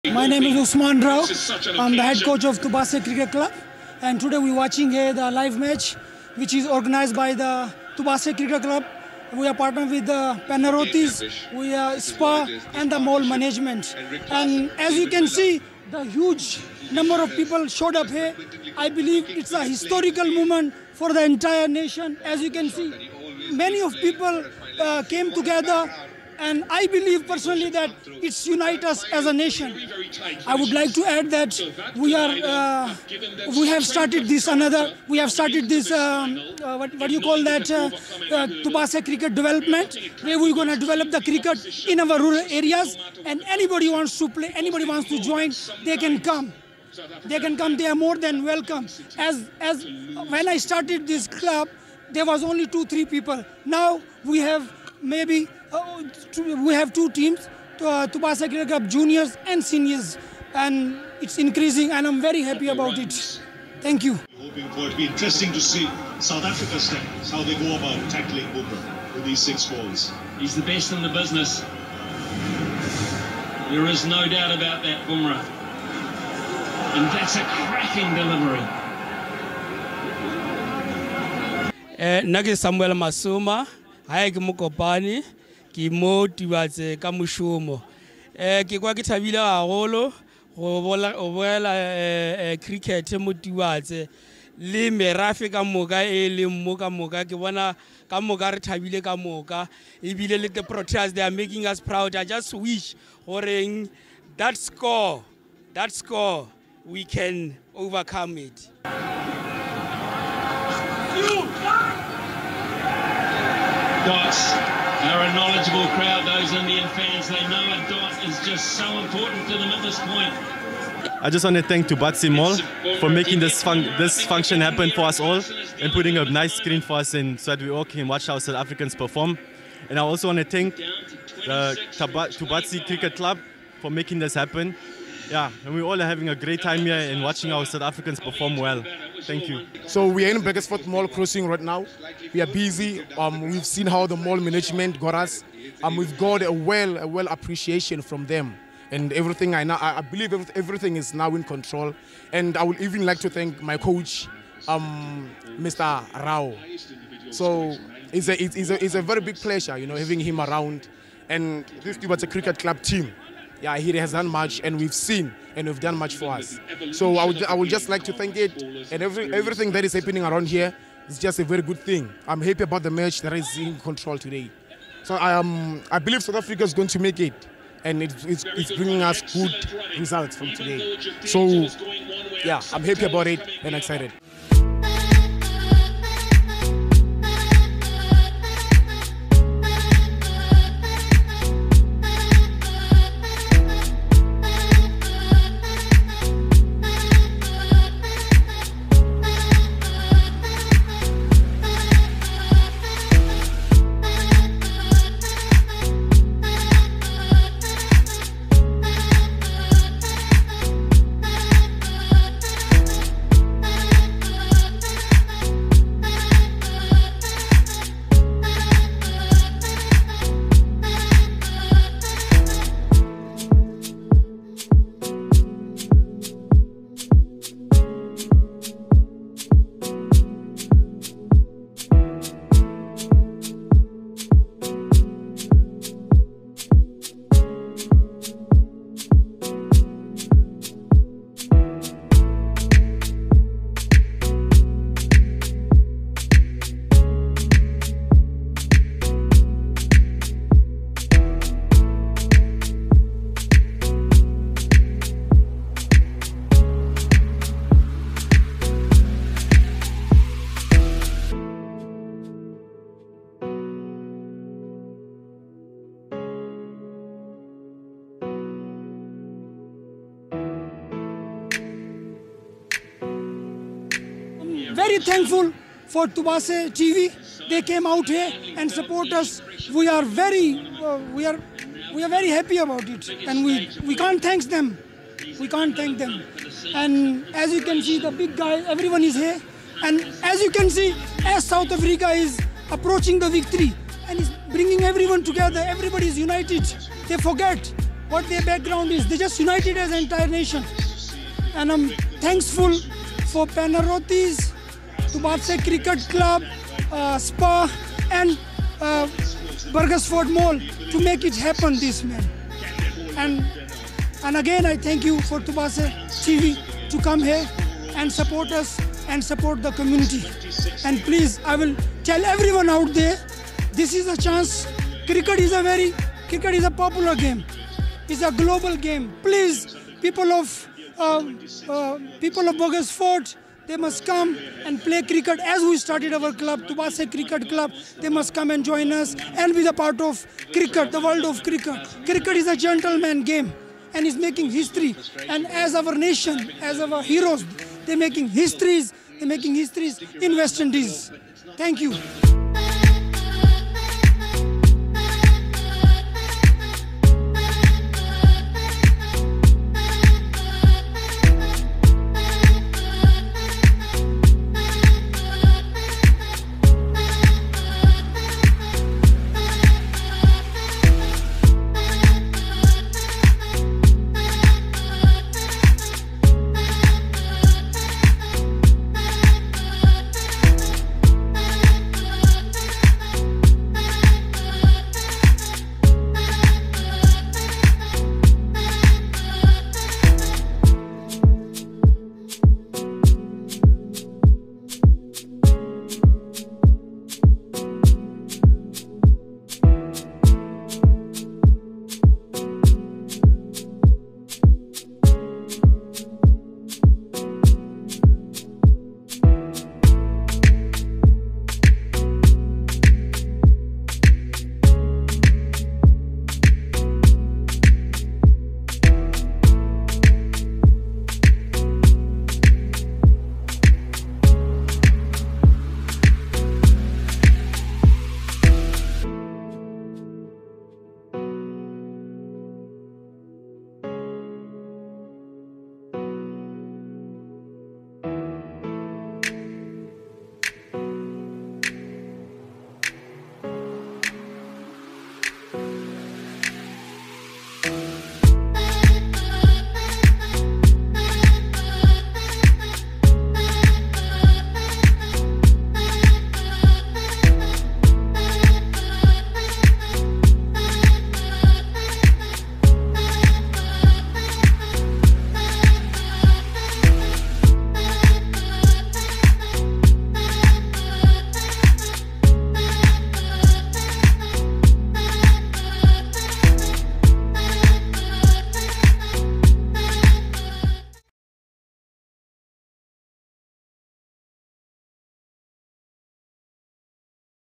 CRICKET LOVERS ERUPT WITH JOY CHEERING FOR THE PROTEAS AT T20 WORLD CUP FINAL LIVE SCREENING EVENT.
A BIG SHOUT OUT TO EVERYONE WHO TUNED IN.

Usmaan Ayub Rao, head coach for the Tubatse Cricket Club told Tubatse TV that the club priority has always been to create a safe, fun environment for the local youth, while offering an opportunity for the kids to learn about the gentleman sport, but also gain the discipline and respect that team sport offers.